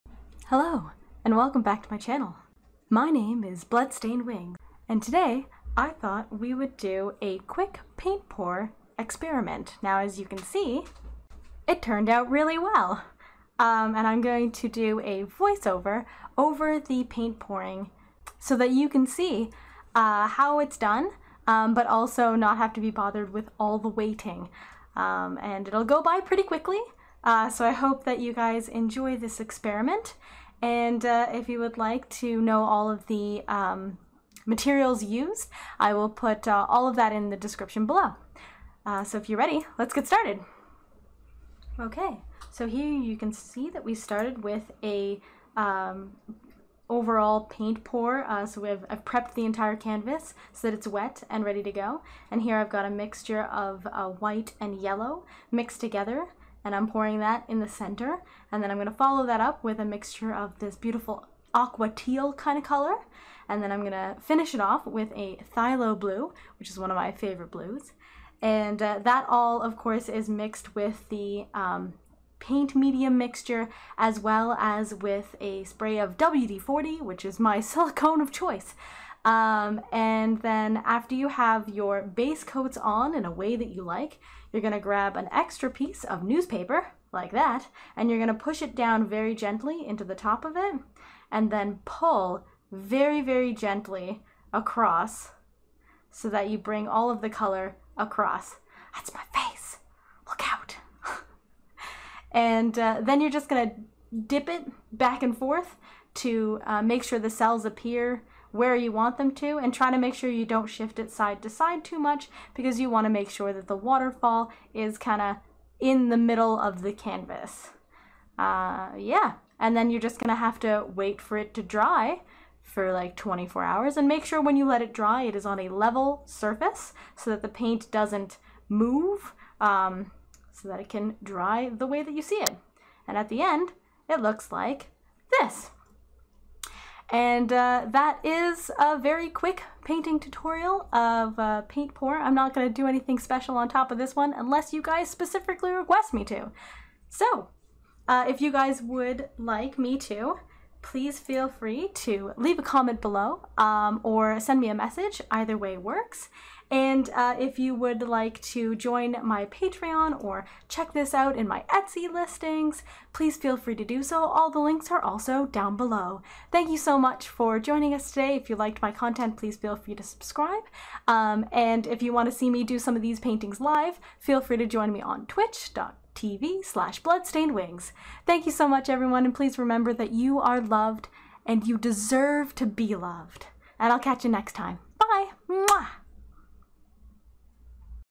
Paint Pouring a Waterfall
This is just a quick voice over of a paint pour technique I tried for the first time.

Materials Used:
-For the paint pouring I used a mixture of 75% elmers glue to 25% water
-Acrylic paint (white, yellow, turquoise, blue and black)
-WD40